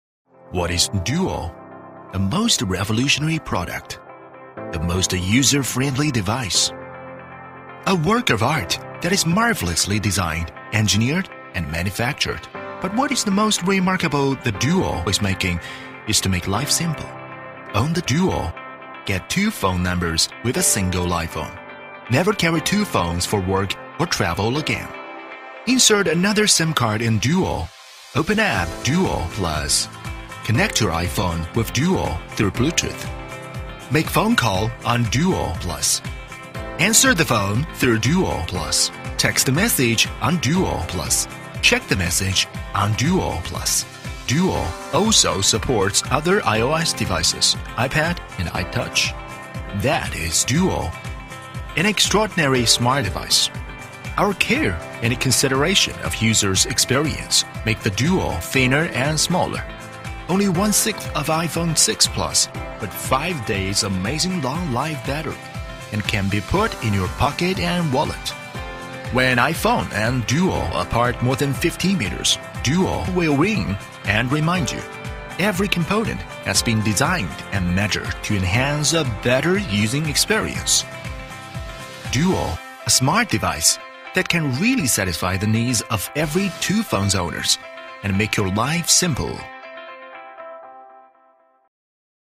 DUOSIM - iPhone Dual Sim Bluetooth Adapter (for iOS device)
DUOSIM-A1, you can use two SIM cards at the same time with for your iPhone. Also perfect solution enabling for iPad/iPod touch to have the phone conversation and texting/receiving messages function.

Official apps: DUO plus is exclusive app that is designed specifically for use with this dual SIM adapter and available in app store

Selfie functions: selfie shutter is available in DUOSIM-A1 to use the remote selfie function, capturing the photos in any time

Smart security: for iPhone alarm will turn if you are more than 10 meters from the DUOSIM-A1 so you do not have to worry about your gadgets left behind or lost

Small and thin design: product has been designed and small and thin as it can with the thickness only 5.5mm and is suitable to put into the wallet or pocket

Product specifications:

1.Product name: DUO SIM-A1 iPhone Bluetooth dual SIM card adapter
2.Long standby time: 7 hours talk time, standby time up to 150 hours
3.Bluetooth effective distance: ≦10m
4.Battery capacity: 440mAh
5.Software supports: iOS 7.0 and above
6.Network: GSM
7.One key answer function
8.Anti-lost (lost alarm)
9.Photo/selfie remote control function
10.SIM card: Nano SIM